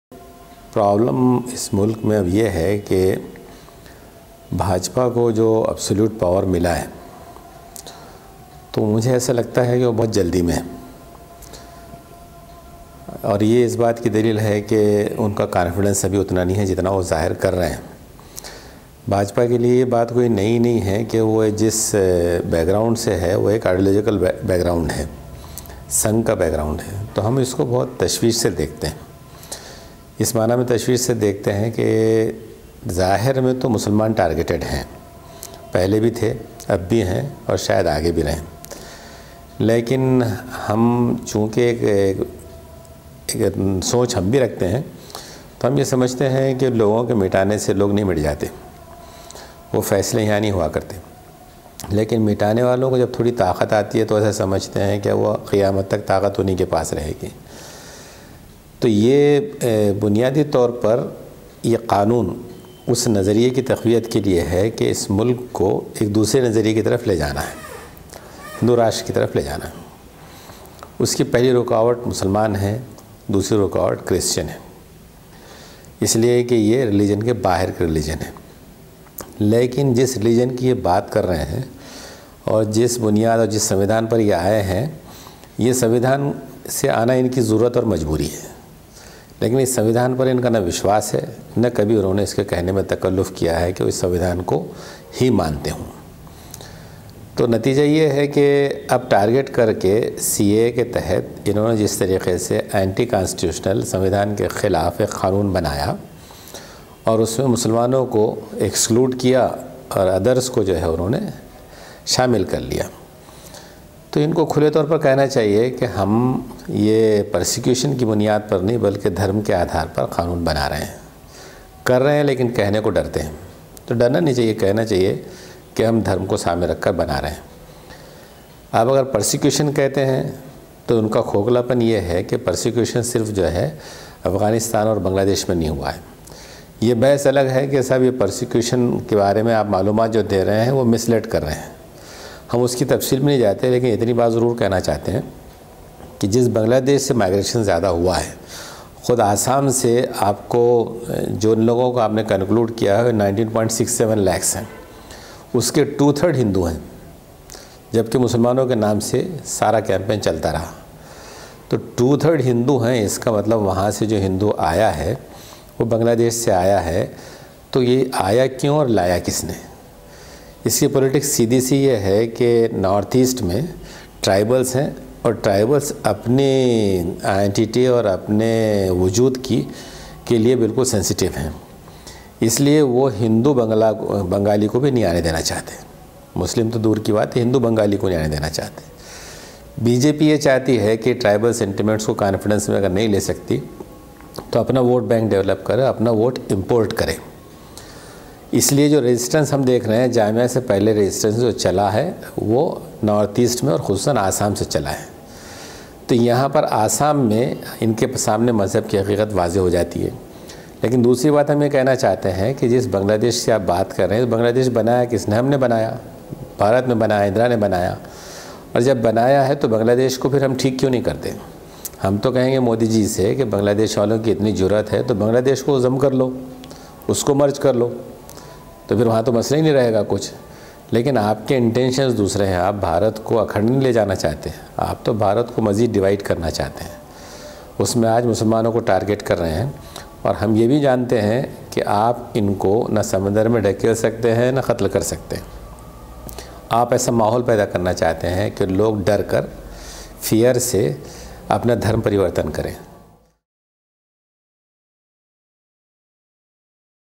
बीजेपी अब डरने क्यों लगी है…?
बीजेपी अब डरने क्यों लगी है…?  बता रहे हैं युनाइटेड अगेंस्ट सीएए एंड एनआरसी से जुड़े मुजतबा फ़ारूक़...